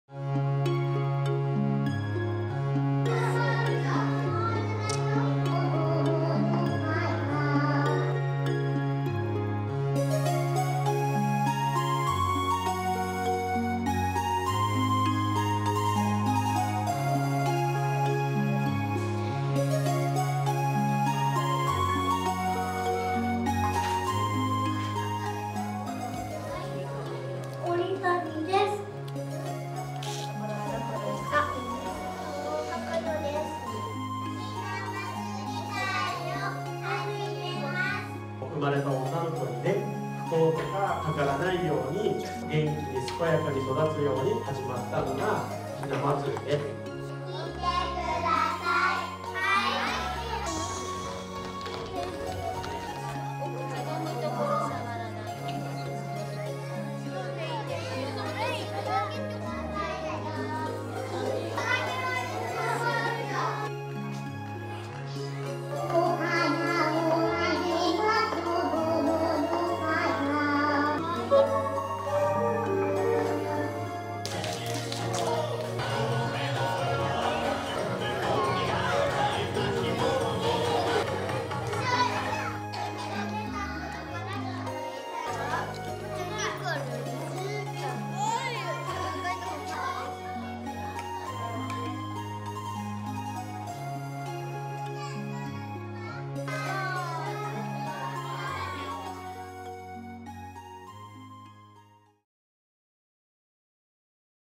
【世界遺産平泉】NEWS#56 もうすぐひなまつり_H28.3.2up
長島保育所で3月1日『ひなまつりかい』が行われ2歳から5歳児55人が参加しました。ひなまつりかいではクラスごとに雛人形の紹介をしたほか、音楽に合わせて踊りを踊ったりしてたくさんの笑顔に包まれました。